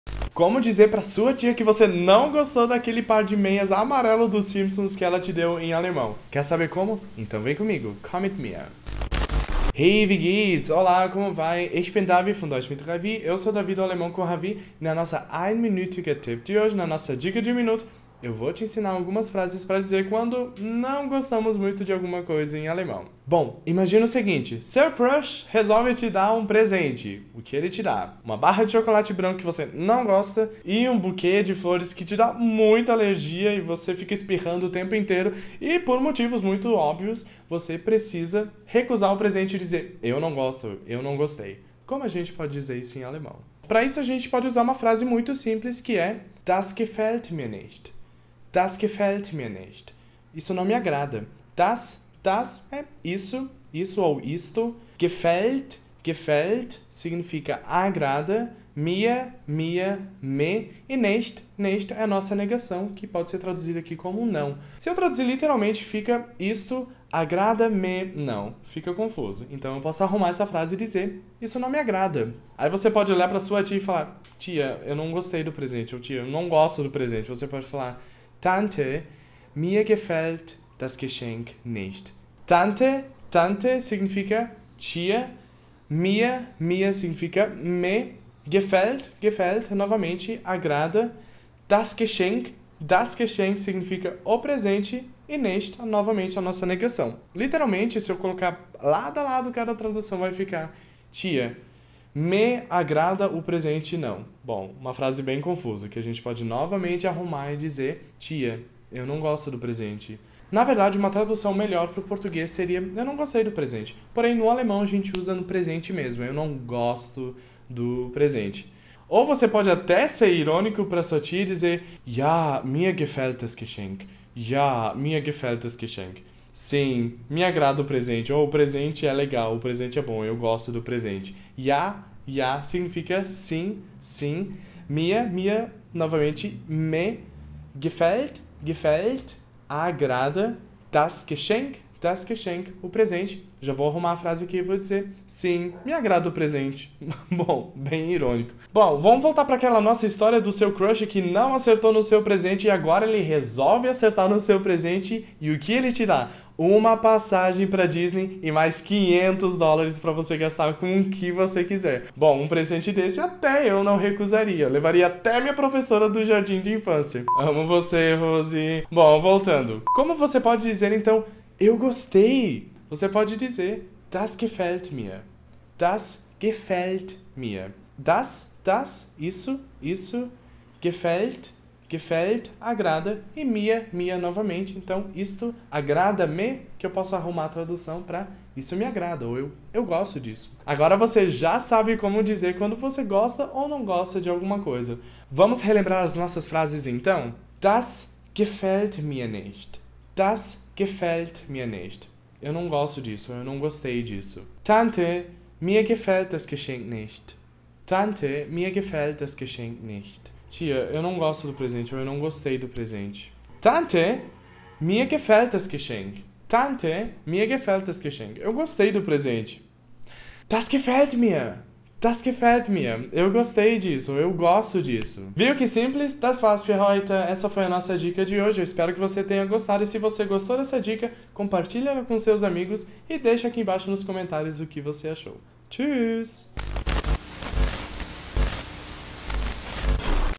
Falando sobre seus gostos em alemão
Quer aprender a falar pra sua tia, em alemão, que você não gostou do par de meias amarelas que ela te deu? O prof Davi te ensina!

Hallo, wie geht’s? Você com certeza já passou por uma situação em que ganhou um presente de que não gostou muito e não soube como dizer isso pra quem te presenteou, certo? Na dica de hoje, o prof Davi vai te ensinar a dizer isso em alemão!

Para falar que alguma coisa não te agrada, você pode usar a seguinte estrutura:

🇩🇪 Das gefällt mir nicht.
🇧🇷 Isso não me agrada.

E, se você quiser falar especificamente sobre o presente da sua tia, pode dizer:

🇩🇪 Tante, mir gefällt das Geschenk nicht.
🇧🇷 Tia, o presente não me agrada.

Coitada da sua tia, né? 🤣

Mas se, por outro lado, você ganhar uma viagem pra Disney mais 500 dólares, com certeza vai gostar do presente! Então, você pode dizer:

🇩🇪 Das gefällt mir!
🇧🇷 Eu gostei/gosto!

E você, já ganhou algum presente que não te agradou? Conte pra gente nos comentários! Em alemão, claro!